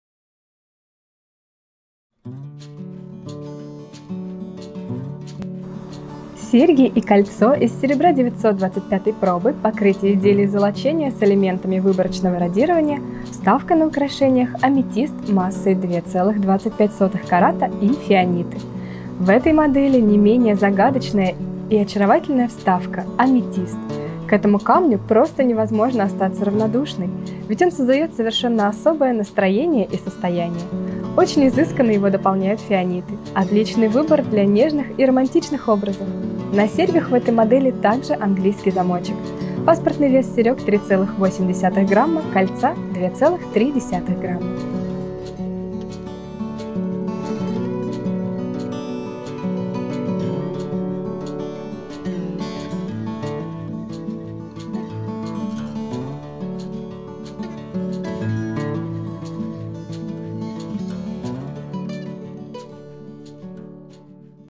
Серьги 33110163ДАм и Кольцо 23110163ДАм - комплект с полудрагоценными вставками!
Покрытие: золочение, выборочное родирование
Вставка: Аметист массой 2,25 карата, фианиты

В этой модели  не менее загадочная и очаровательная вставка - аметист! К этому камню просто невозможно остаться равнодушной, ведь он создаёт  совершенно особое настроение и состояние! И очень изысканно его дополняют фианиты. Отличный выбор для нежных и романтичных образов. На серьгах в этой модели английский замочек.

Паспортный вес серёг - 3,8 грамма, кольца - 2,3 грамм